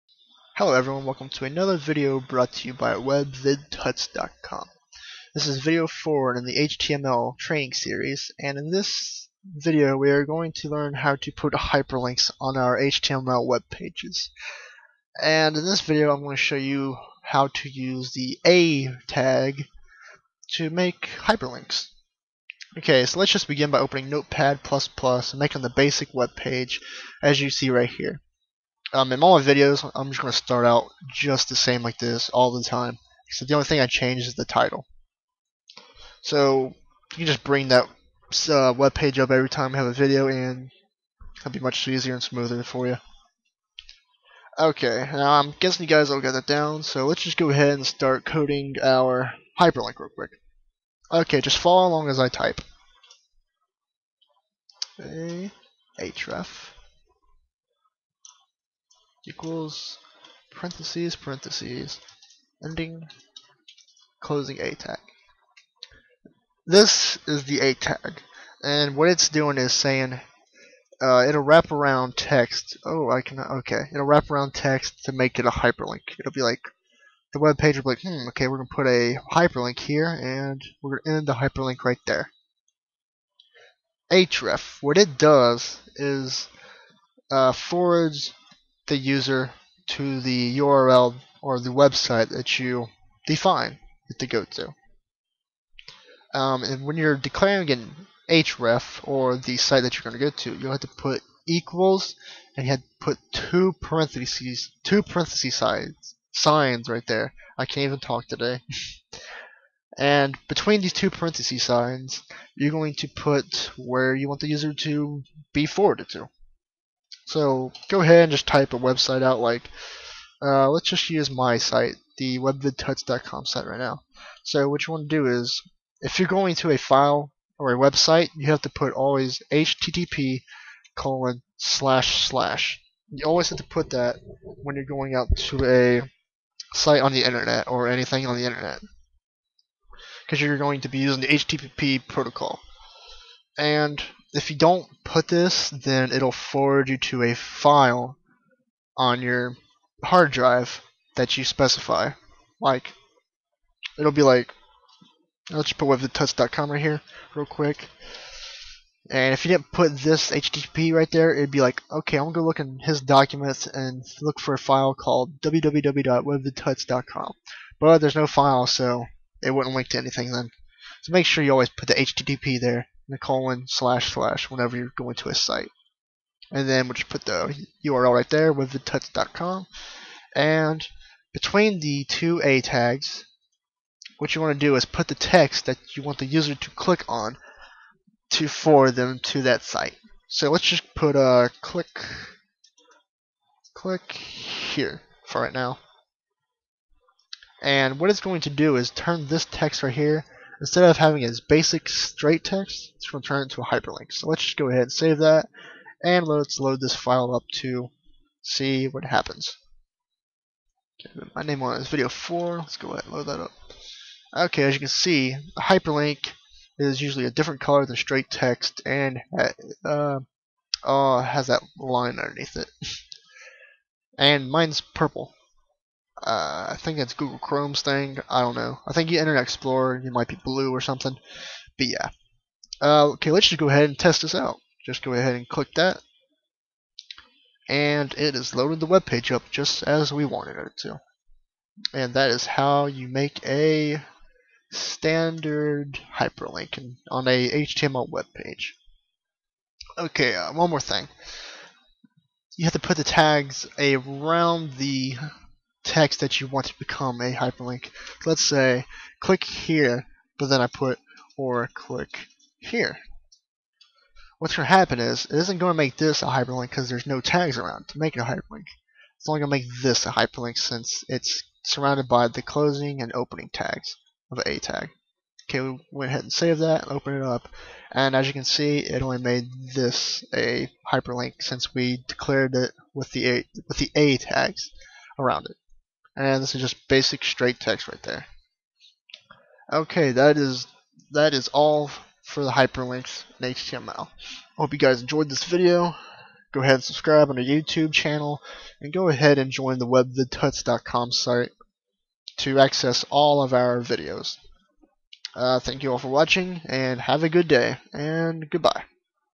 HTML Training - 4 - Hyperlinks
In this video of the HTML Training series we will be learning how to put hyperlinks to other sites on your webpages to forward users to other webpages or websites that you define for them.